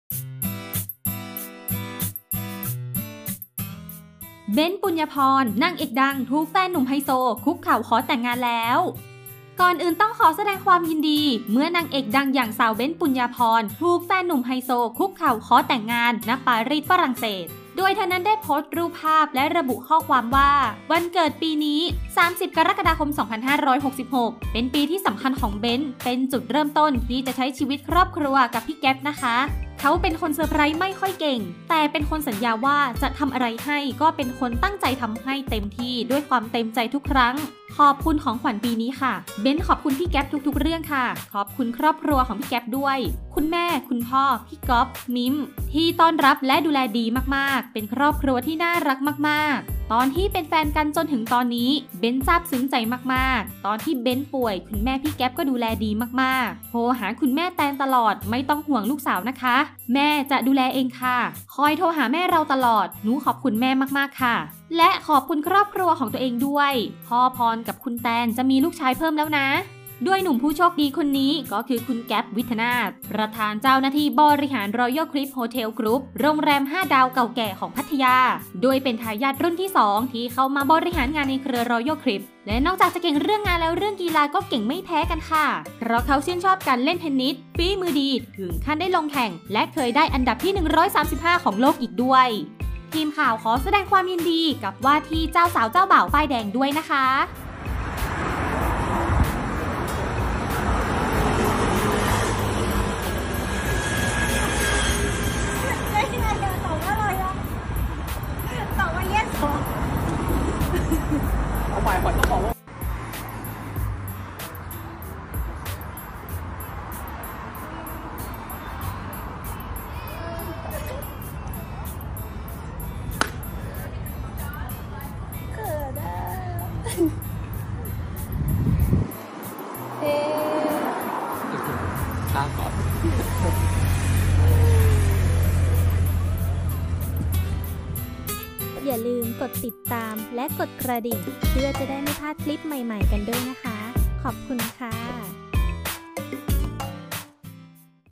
กรี๊ด นางเอกสาว เบนซ์ ปุณยาพร ถูกขอแต่งงาน แฟนรวยดีกรีเจ้าของโรงแรมดัง
งานปัง ความรักก็ชื่นมื่นลงตัวสุดๆสำหรับนักแสดงสาวชื่อดัง "เบนซ์ ปุณยาพร พูลพิพัฒน์" แม้ว่าเราจะไม่ค่อยเห็นโมเมนต์สวีทของสาวเบนซ์กับหวานใจนักธุรกิจหนุ่มดีกรีเจ้าของโรงแรมดังเก่าแก่ของพัทยาอย่าง "แก๊ป วิทนาถ วรรธนะกุล"
ทั้งคู่รักกันมา 4 ปี ล่าสุดไฮโซแก๊ปได้ทำเซอร์ไพรส์คุกเข่าขอแต่งงานที่ฝรั่งเศส โดยสาวเบนซ์โพสต์ภาพและคลิปโมเมนต์นี้ผ่านอินสตราแกรมส่วนตัว benzpunyaporn พร้อมความในใจเผยว่า "วันเกิดปีนี้ 30/07/66 เป็นปีสำคัญของเบนซ์ เป็นจุดเริ่มต้นที่จะใช้ชีวิตครอบครัวกับพี่แก็ปนะคะ เค้าเป็นคนเซอร์ไพรส์ไม่ค่อยเก่ง แต่เป็นคนสัญญาว่าจะทำอะไรให้ก็เป็นคนตั้งใจทำให้เต็มทีด้วยความเต็มใจทุกครั้ง ขอบคุณของขวัญปีนี้ค่ะ เบนซ์ขอบคุณพี่แก็ปทุกๆเรื่องค่ะ ขอบคุณครอบครัวของพี่แก๊ปด้วย คุณแม่ คุณพ่อ พี่กอล์ฟ มิ้ม ที่ต้อนรับและดูแลดีมากๆ เป็นครอบครัวที่น่ารักมากๆตอนที่เป็นแฟนกันจนถึงตอนนี้ เบนซ์ซาบซึ่งใจมากๆตอนที่เบนซ์ป่วยคุณแม่พี่แก็ปดูแลดีมากๆ โทรหาคุณแม่แตนตลอดไม่ต้องห่วงลูกสาวนะคะ แม่จะดูแลเองค่ะ ค่อยโทรหาแม่เราตลอด นู๋ขอบคุณแม่มากๆค่ะ และขอบคุณครอบครัวของตัวเองด้วย พ่อพรกับคุณแตนจะมีลูกชายเพิ่มแล้วนะ